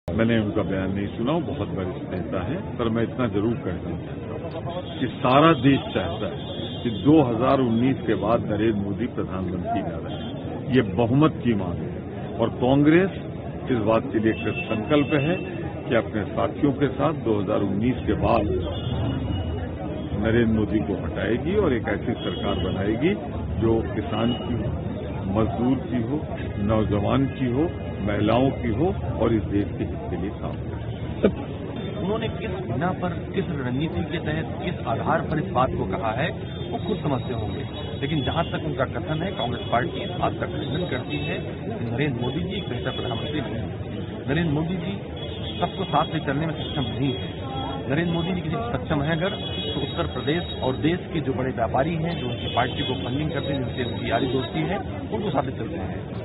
सपा सुप्रीमो के बयान पर बोले कांग्रेसी देश नहीं चाहता मोदी फिर पीएम बनें
लखनऊ : सपा सुप्रीमो मुलायम सिंह यादव के लोकसभा में दिए गए बयान से राजनीतिक गलियारों में हलचल मच गई है. कांग्रेसी नेताओं ने इसपर प्रतिक्रया देते हुए कहा कि मुलायम सिंह चाहे जो भी कहें, लेकिन देश नहीं चाहता कि नरेन्द्र मोदी 2019 के बाद प्रधानमंत्री रहें.